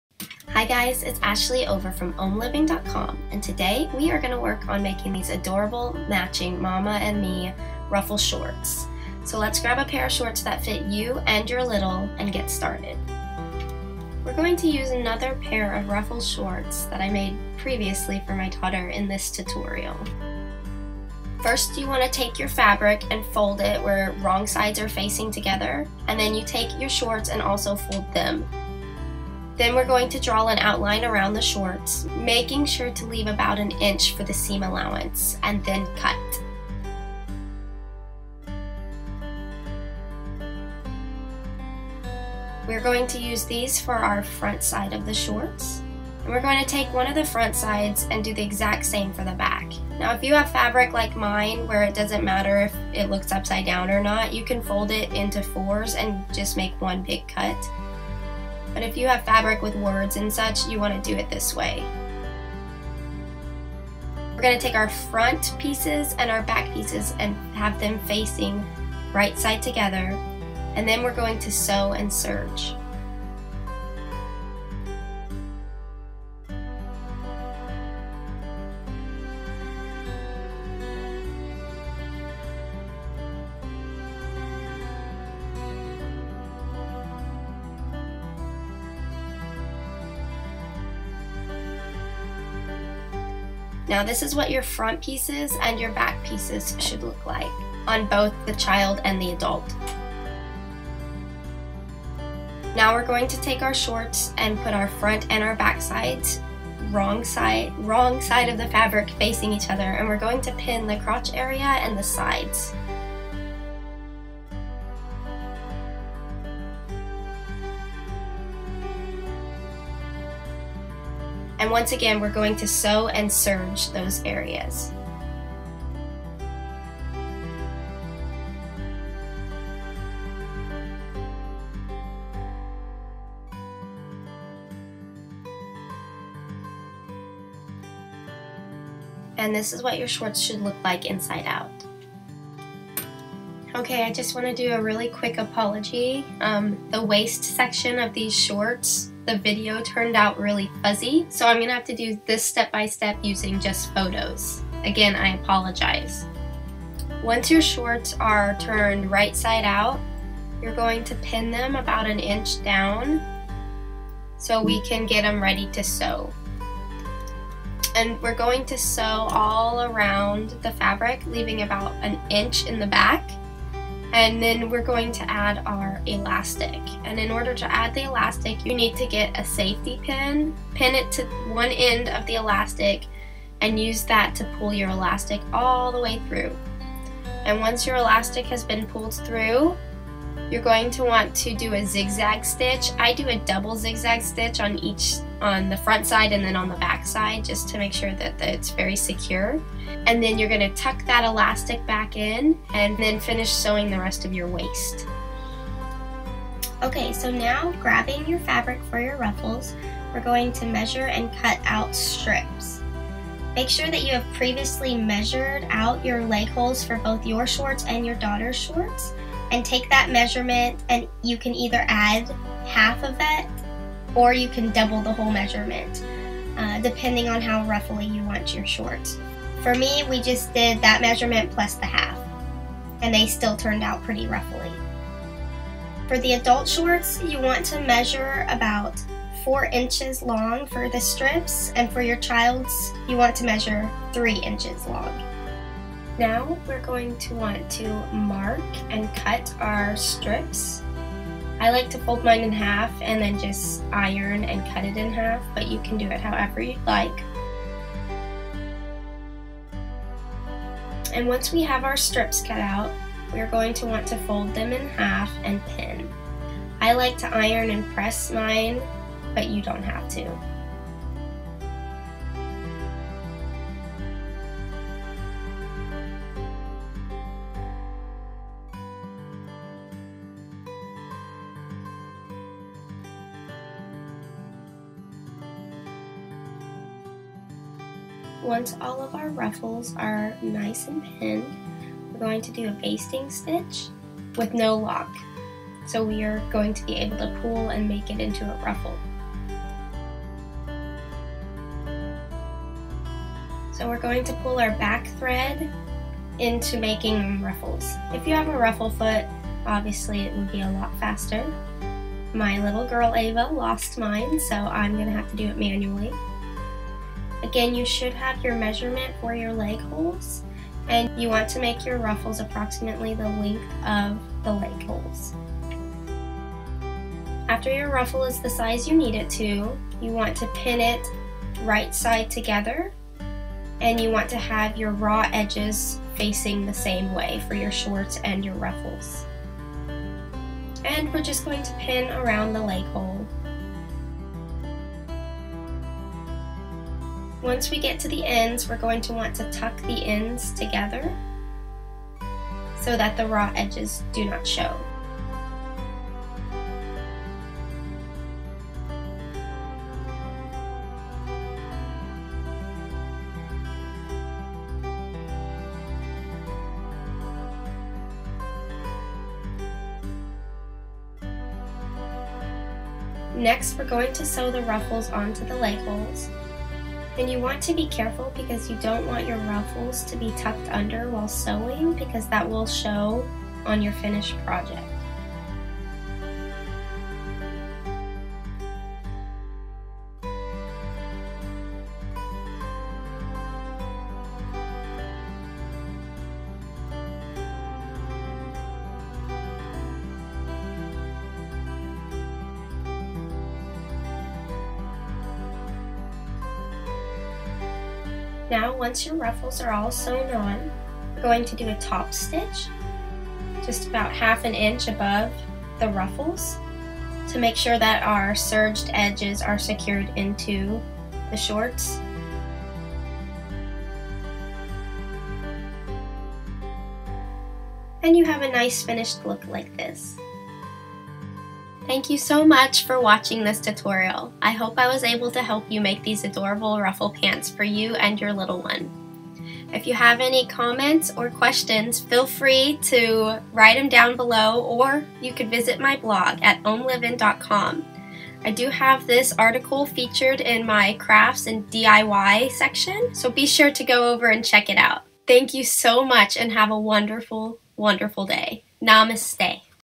DIY Mama & Me matching Ruffle Shorts Tutorial
Be sure to turn heads with these adorable matching ruffle shorts. A quick tutorial on how to make a matching set of ruffle shorts for you & your daughter.